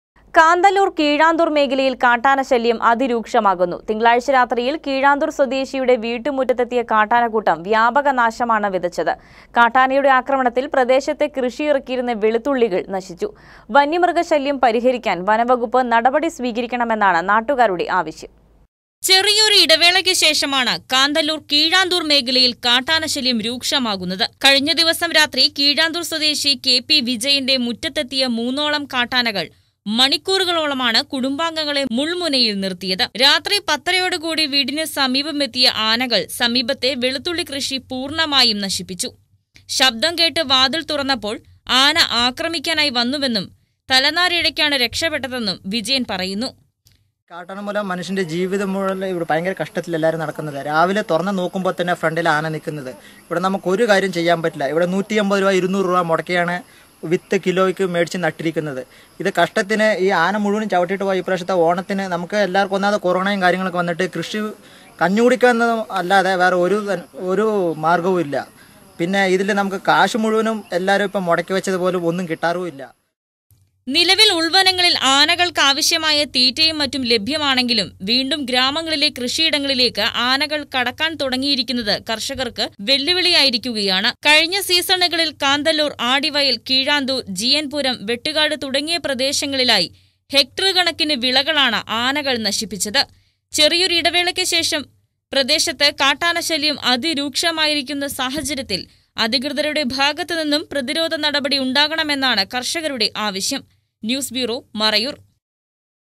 കാന്തല്ലൂര്‍ കീഴാന്തൂര്‍ മേഖലയില്‍ കാട്ടനാശല്യം അതിരൂക്ഷം..
കാന്തല്ലൂര്‍ കീഴാന്തൂര്‍ മേഖലയില്‍ കാട്ടനാശല്യം അതിരൂക്ഷമാകുന്നു. തിങ്കളാഴ്ച രാത്രിയില്‍ കീഴാന്തൂര്‍ സ്വദേശിയുടെ വീട്ടുമുറ്റത്തെത്തിയ കാട്ടാനക്കൂട്ടം വ്യാപക നാശമാണ് വിതച്ചത്.കാട്ടാനയുടെ ആക്രമണത്തില്‍ പ്രദേശത്ത് കൃഷിയിറക്കിയിരുന്ന വെളുത്തുള്ളികള്‍. നശിച്ചു.വന്യമൃഗശല്യം പരിഹരിക്കാന്‍ വനംവകുപ്പ് നടപടി സ്വീകരിക്കണമെന്ന് നാട്ടുകാര്‍ ആവശ്യപ്പെട്ടു.